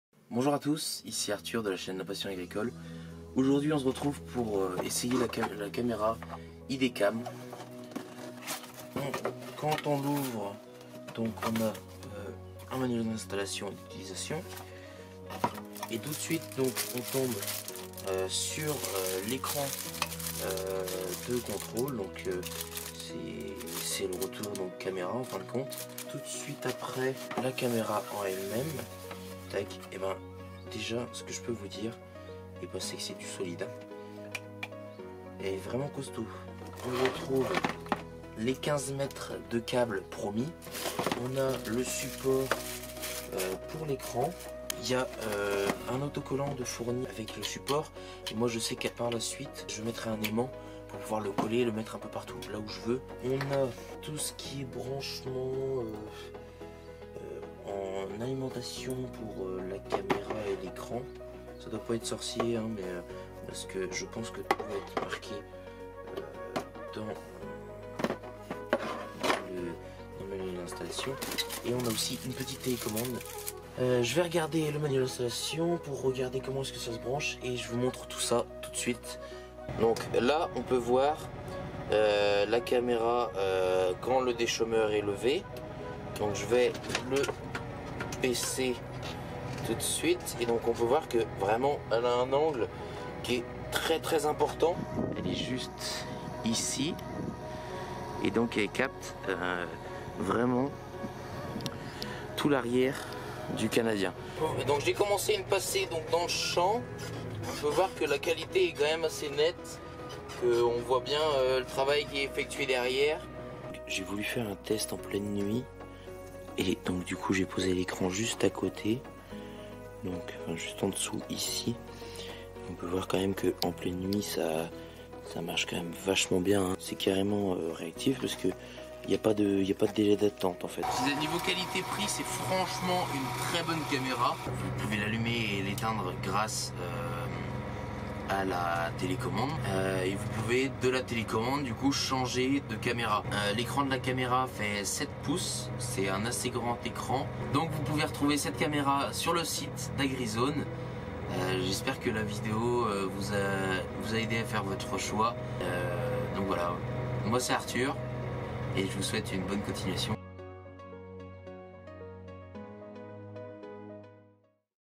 Kit caméra de recul tracteur- Montage et avis
Retrouvez l'avis et le test vidéo d'un client Agrizone utilisant le kit caméra de recul 7" ID CAM pour cabine tracteur.